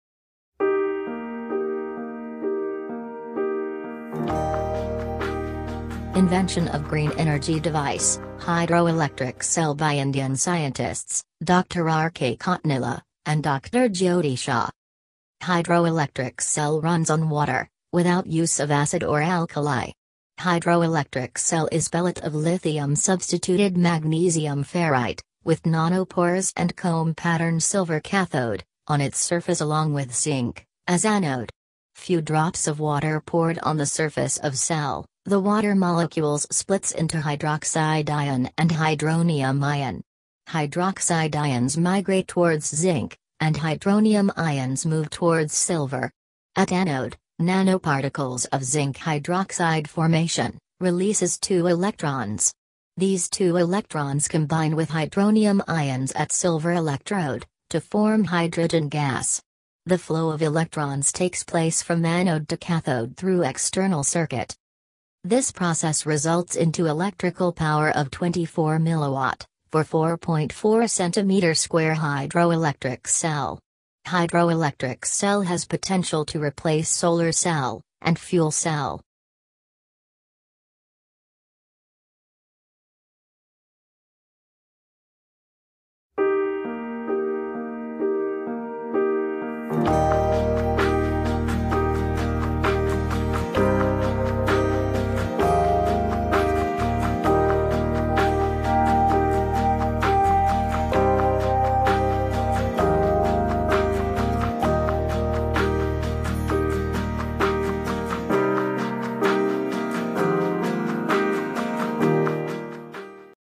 Fan and LEDs runs on water drops Incredible Invention!!! Hydroelectric cell
This video shows the water dissociation mechanism by the material and capturing of dissociating ions by electrodes. By water ionization hydroelectric cell generates electric current and voltage. This cell combines material science, nanoscience and electrochemistry. First research paper has been published by Indian Scientists.